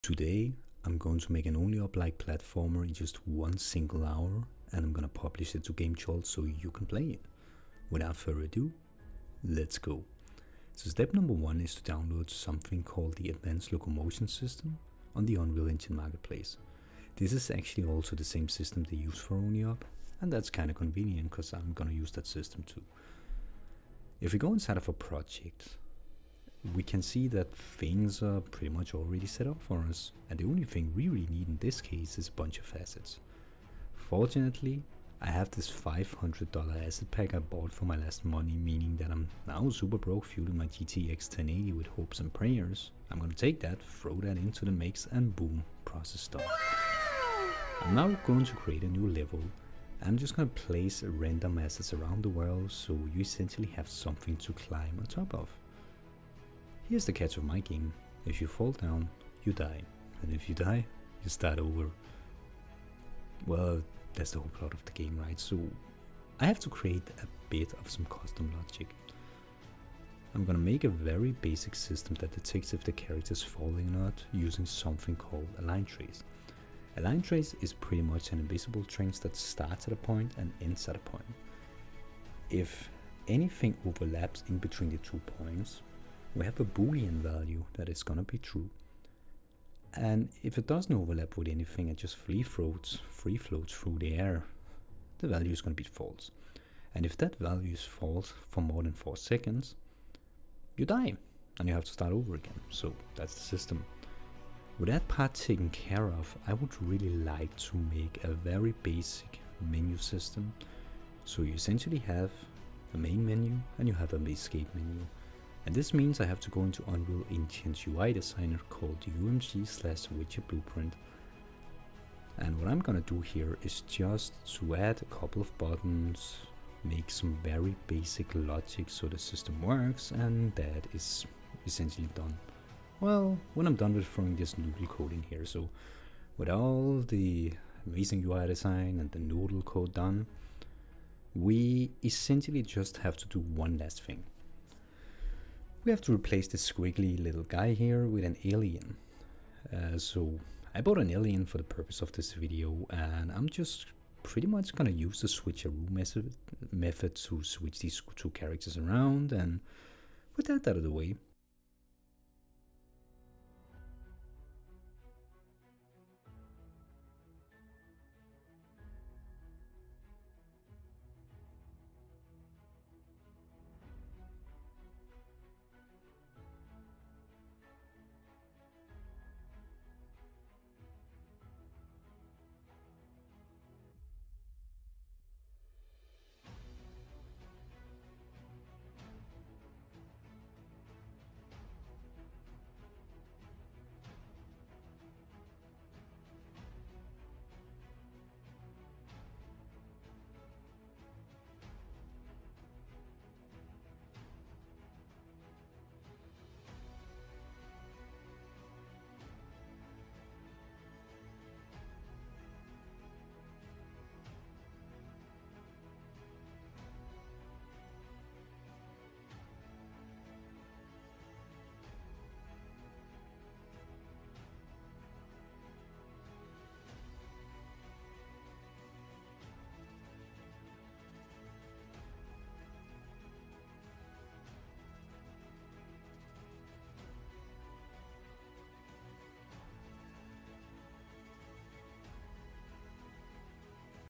I made Only up! in 1 HOUR!
I made a Only Up like game in just one hour because why not?
I used the same system as the original and just replaced the blue guy with something a bit more appropriate and dropped in some assets.